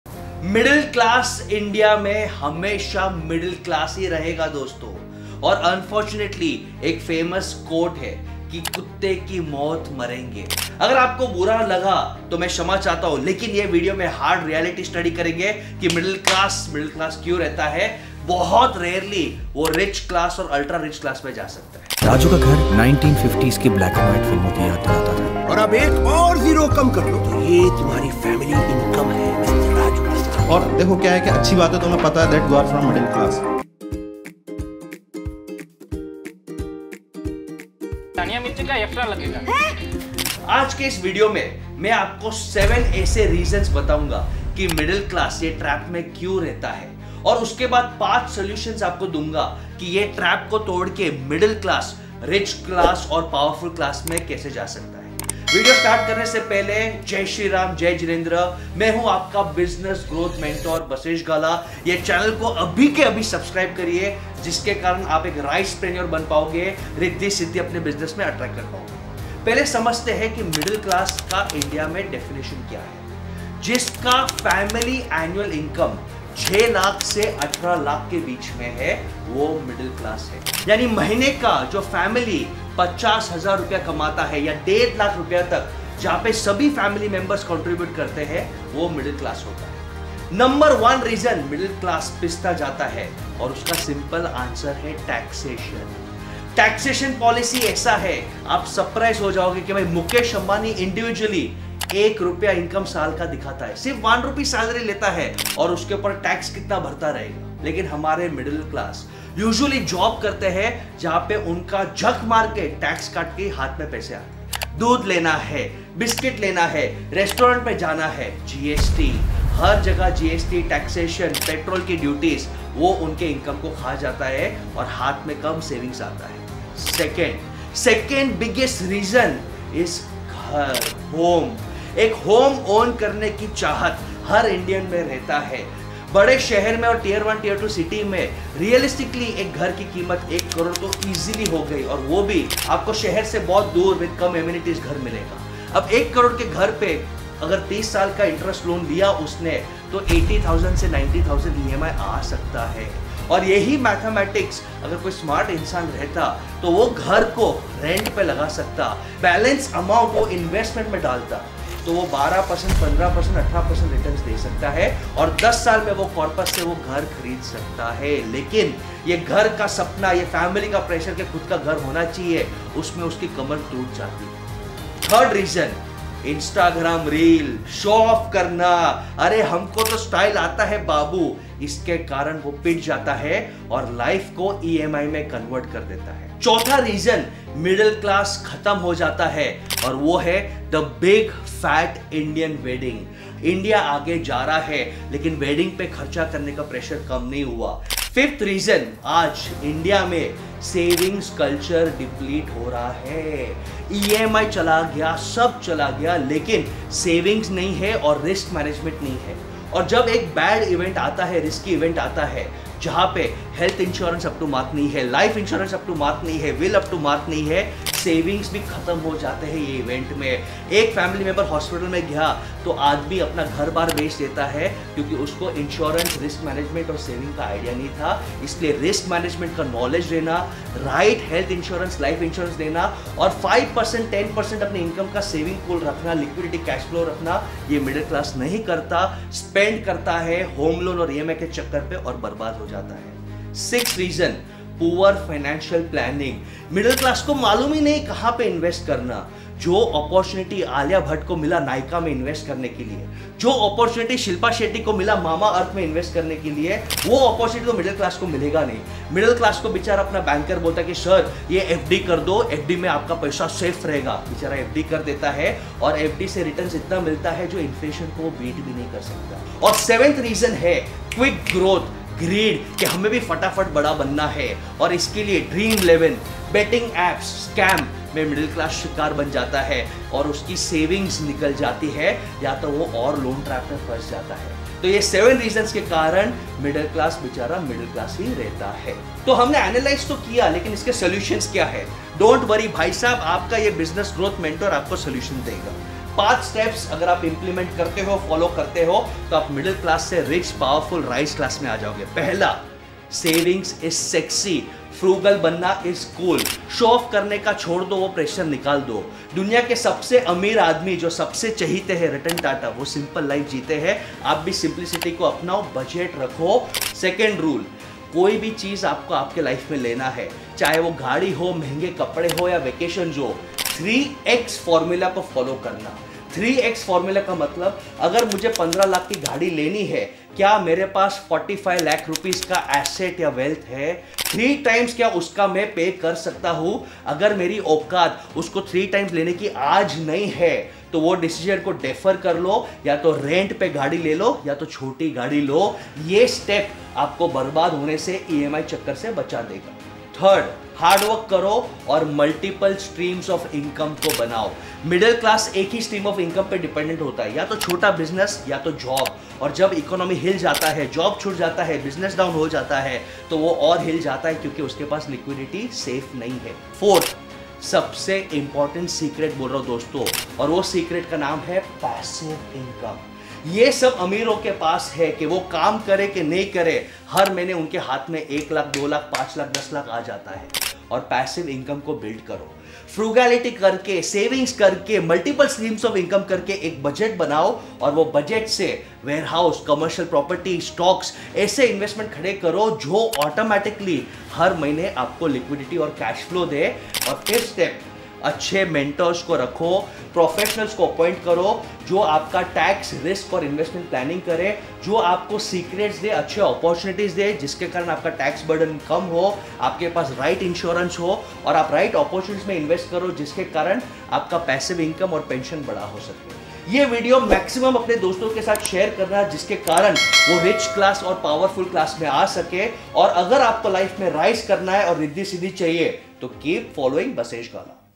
Why Middle-Class Will Never Become Rich: Shocking Reality
Are you part of the middle class and wondering why financial success seems out of reach? This eye-opening video reveals the harsh truths behind why the middle class is struggling more than ever.

Discover the 7 major factors contributing to the decline of middle-class wealth and what you can do to navigate these hurdles. This is a must-watch for anyone concerned about their financial future!

So Watch the full video, to know the hidden traps that keep the middle class stuck and uncover the 7 major reasons why the middle class is becoming poorer day by day.

1. Understand how you're stuck in a Middle-Class Trap
2. The problem with buying Home
3. Receive insights on Life's Important areas like Finance, Marriage, and Career.
4. Poor Financial Planning
5. Steps to join rich and powerful people

Timeslots-
00:00 - 01:08 = Introduction
01:09 - 02:07 =  Who belongs to Middle-Class Family?
02:08 - 02:57 = The problem with buying Home
02:58 - 03:05 = The Show off on social media
03:06 - 03:17 = Problem with Indian Wedding
03:18 - 04:02 = The Reality of Savings in India
04:06 - 04:28 = Poor Financial Planning
04:29 - 04:43 = Greed and Quick growth Mentality
04:45 - 06:56 = 5 Steps to join rich and powerful people
06:57 - 07:07 = Outro